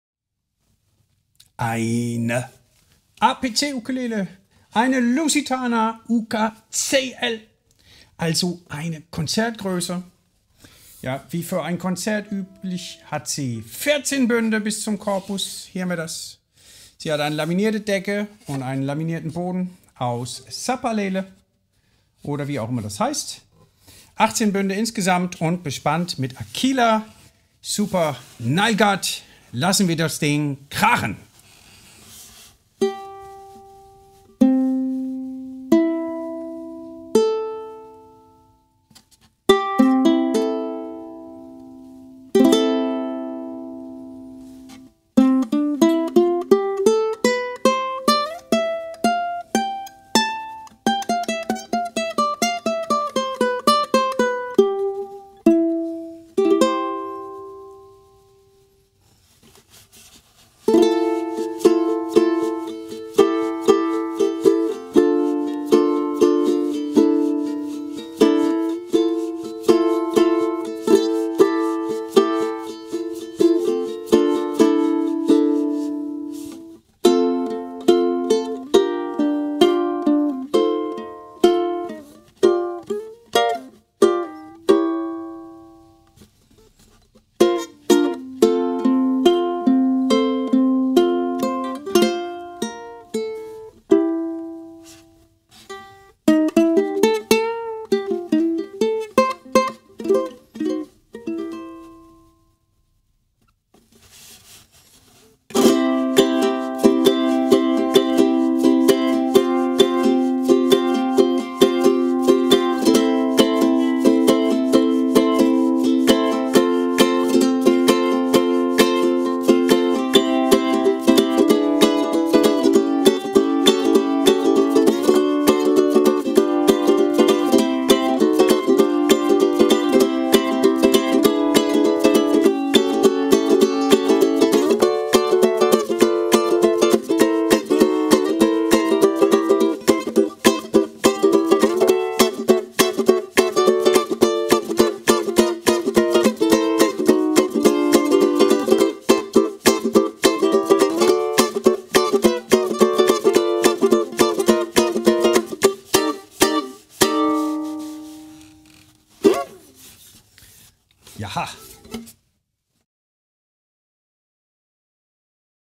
Soundcheck: APC Lusitana Sapele UKCL Konzert für Uke Supply
Uke Supply habe mich gebeten, einige Ukulelen für sie anzuspielen. Bitteschön: die APC Lusitana Sapele UKCL Konzert.